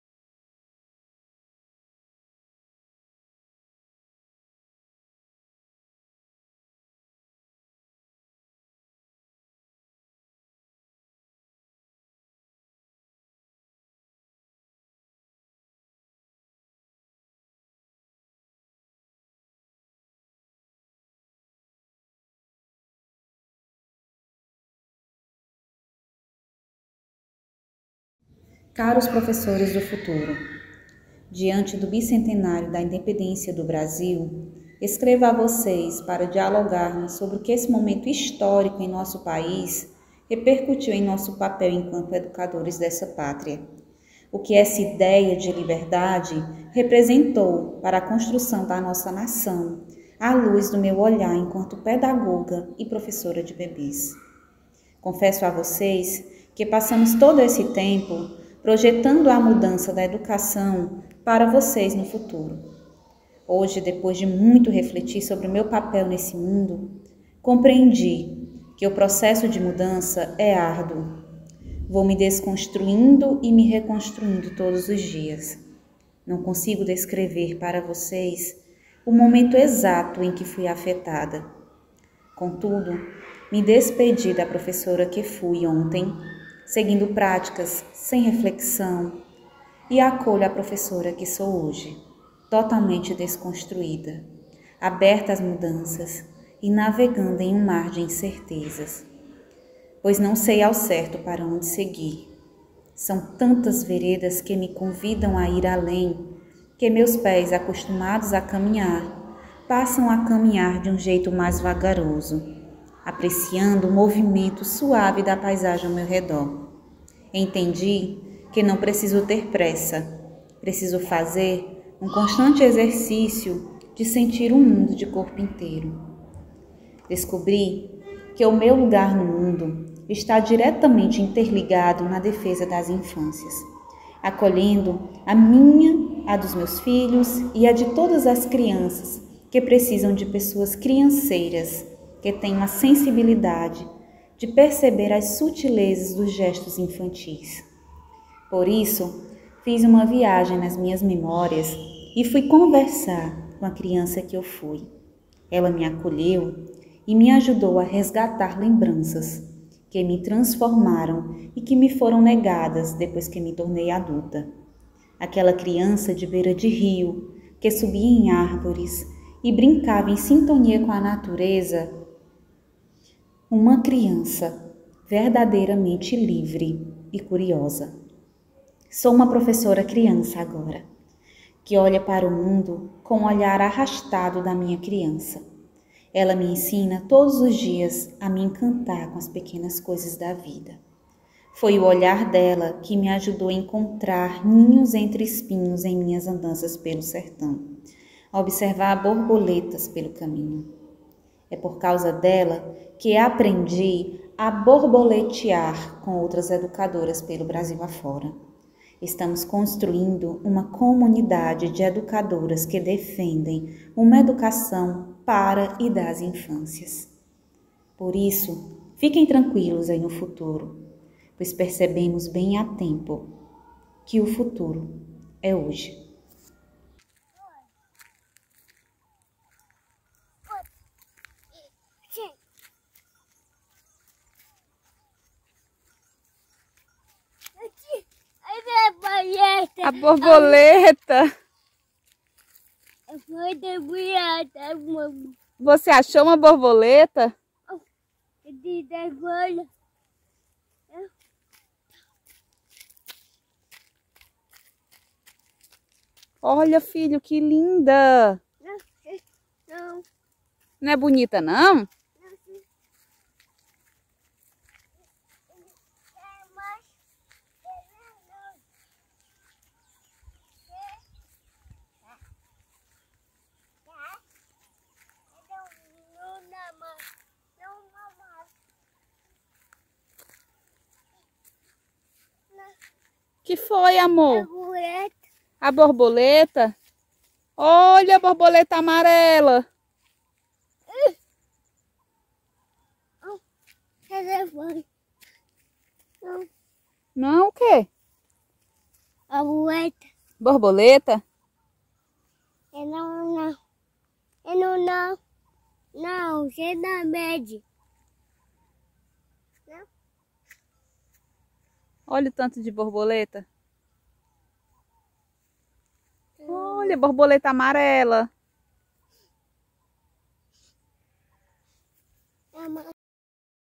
Filme-carta da Tunica para as Professoras do Futuro
Sinopse:
Um olhar sensível e poético sobre as travessias e percursos de uma professora “crianceira”, navegando em um mar de incertezas na busca pela defesa das infâncias diante do Bicentenário da Independência.

Créditos:

Minha gratidão a todos os bebês do CEI Terezinha de Jesus Ponte Aragão que me ensinaram a perceber as sutilezas de ser professora, a todas as famílias que atenciosamente acolheram as minhas propostas durante a pandemia e foram meus olhos e meus ouvidos para observar o brincar das crianças e a se maravilhar com a curiosidade das crianças em descobrir o mundo ao seu redor, obrigada infinitamente.
Ao querido Thiago Rosado por acreditar e me incentivar mesmo quando ficava insegura,
um amigo que tenho muito carinho apesar de nunca ter visto pessoalmente.
Enfim, sigo nas minhas andanças, sempre buscando sentido naquilo que faço.

Professora-Cineasta: Tunica Airles
Imagens e Carta: Tunica Airles
Condução do processo: Thiago Rosado
Montagem: Letícia Pires e Thiago Rosado
Realização:Portal do Bicentenário
Intérprete de Libras:Jane Silva
Designer Gráfico (artes de divulgação): Gabriel de Medeiros Guilherme
Financiamento: Programa de Pós-Graduação da Universidade Federal de Pernambuco(PPGE-UFPE)

O presente trabalho foi realizado com apoio da Coordenação de Aperfeiçoamento de Pessoal de Nível Superior – Brasil (CAPES)  por meio do edital Proap 2/2021-PPGE-UFPE.

2022